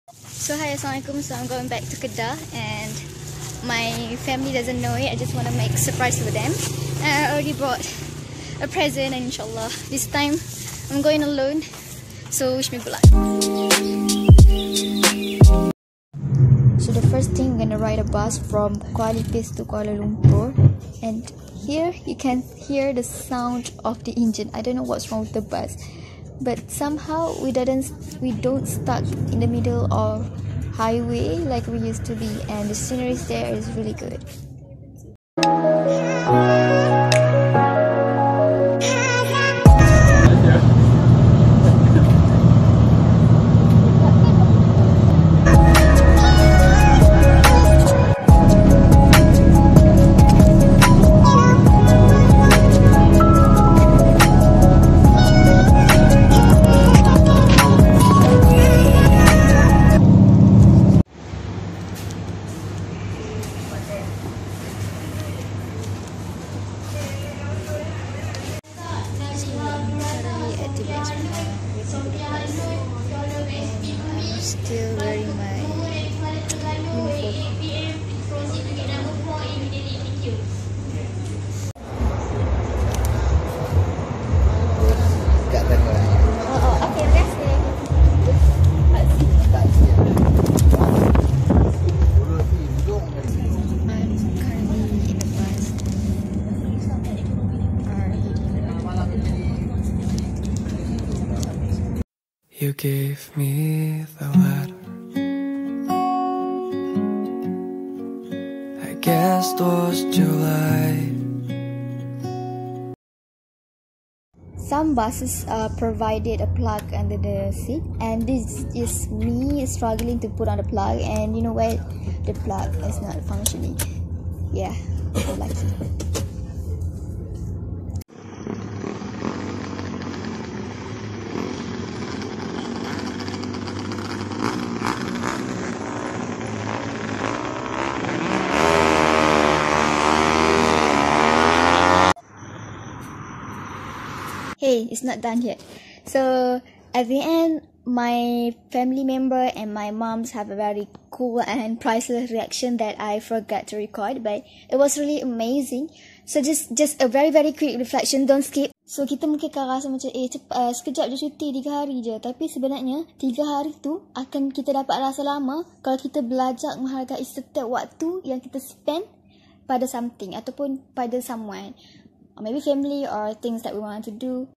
Suprising My Family | Vlog #2
They didn't know I'm heading home. My journey to home take almost a day,  so it make sense if I'm not home frequently.  But since I got five days off,  I'll go home.